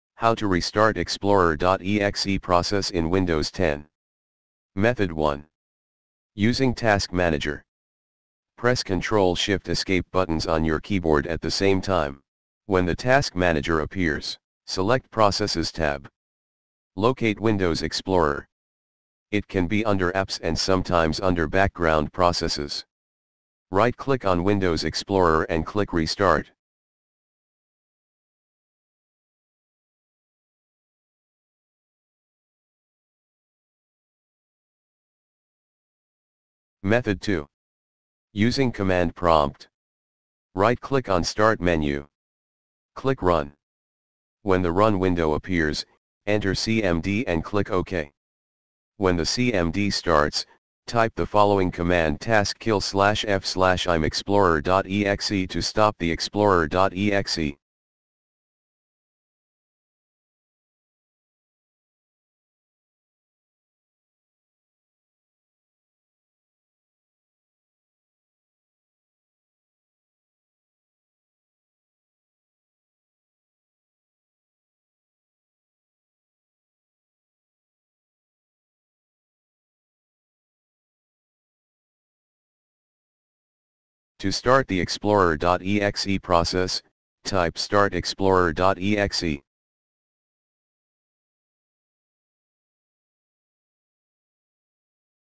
How to Restart explorer.exe Process in Windows 10
You can restart Windows explorer in various ways.
Some of the methods are explained below :

Method 1. Using Task Manager.
Step 1. Press Control Shift Escape Buttons On Your Keyboard At The Same Time.
Step 2. When The Task Manager Appears.
Step 3. Select Processes Tab.
Step 4. Locate Windows Explorer. It Can Be Under Apps And Sometimes Under Background Processes.
Step 5. Right Click On Windows Explorer And Click Restart.

Method 2. Using Command Prompt.
Step 1. Right Click On Start Menu.
Step 2. Click Run.
Step 3. When The Run Window Appears, Enter Cmd And Click Ok.
Step 4. When The Cmd Starts, Type The Following Command "Taskkill /F /Im Explorer.Exe" To Stop The Explorer.Exe.
Step 5. To Start The Explorer.Exe Process, Type "Start Explorer.Exe".